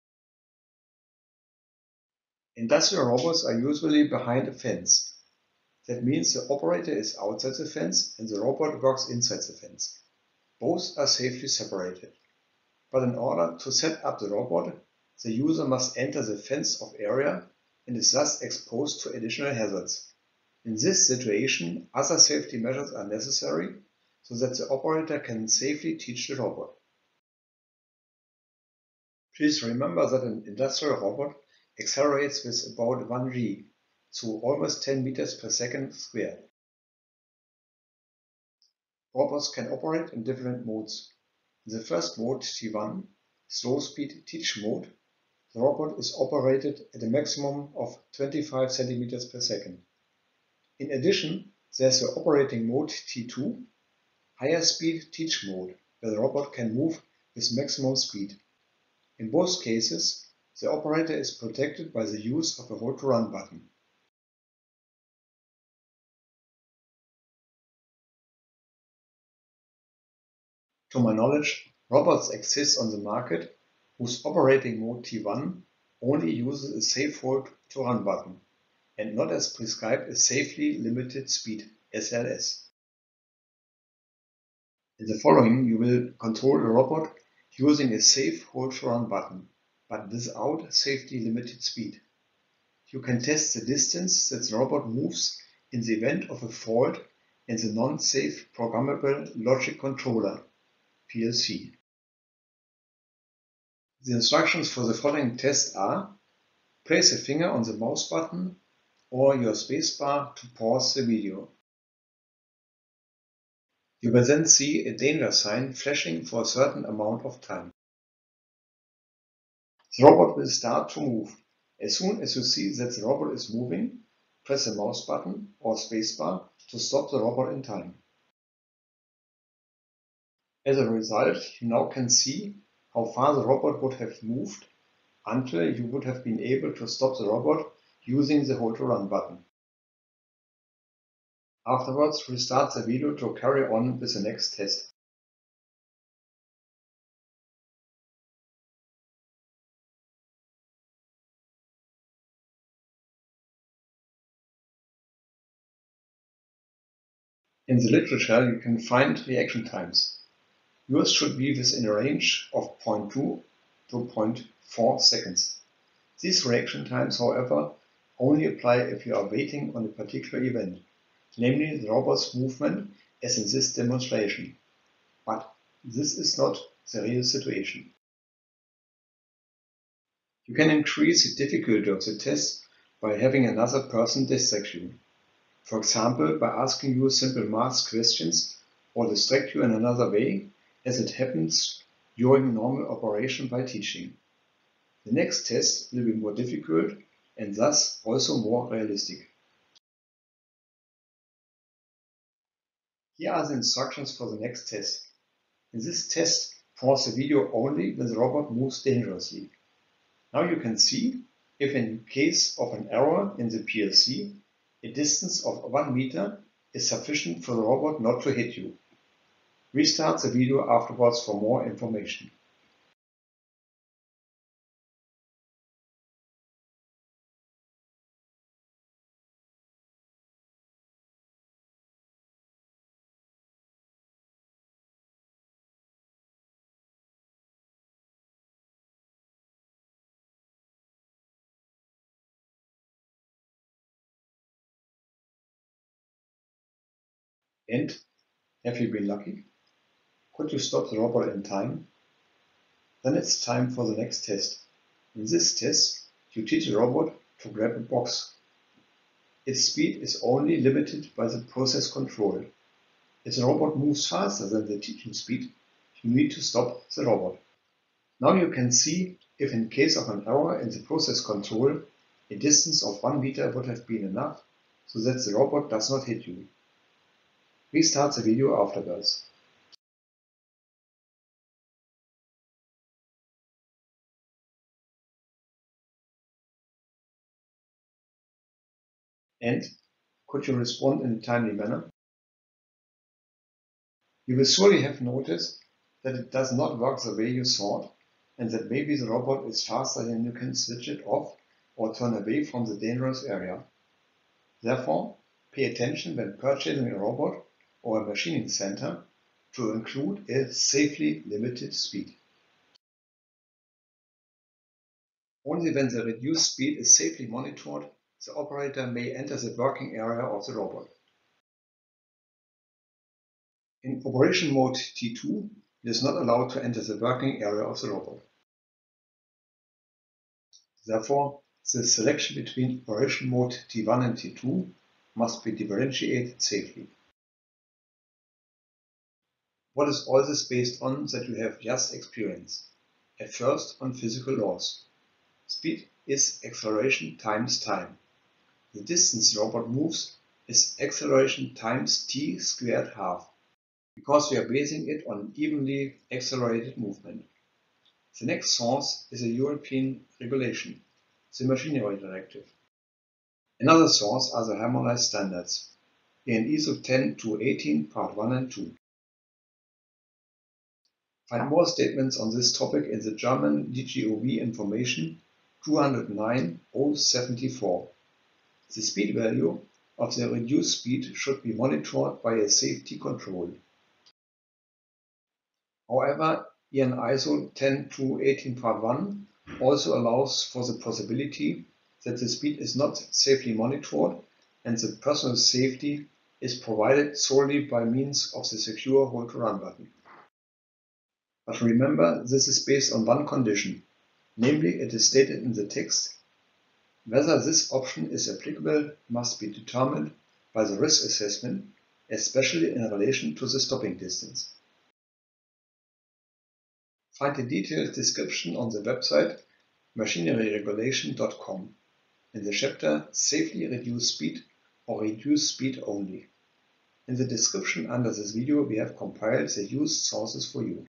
Limiting robot speed in T1 safely?
Annex I no. 1.2.5 requires the installation of a mode selector switch if a machine is capable of several control or operating modes with different protective measures and / or working methods. The mode selector switch allows you to switch between the protective measures that are adapted to the respective operating mode. In addition to the normal operation the setup operation is one of these modes.

In various operating modes, protective measures that are used in normal operation must be regularly switched off. For setup it can be necessary that the operator must access the inner area of the machine, behind the actual protective equipment. He may have to observe the process of the running machine in order to find the optimal setting of the machine, such as: when teaching an industrial robot. Even if only specially trained personnel are used for this phase of life of the machine, the graduated measures according to the concept of integrated safety must be observed here - see Annex I, No. 1.1.2. Annex I of the Machinery Directive.

This video is for introductory general information only. Therefore, it cannot claim to be complete and legally binding. Liability and recourse claims of any kind are excluded to the extent permitted by law.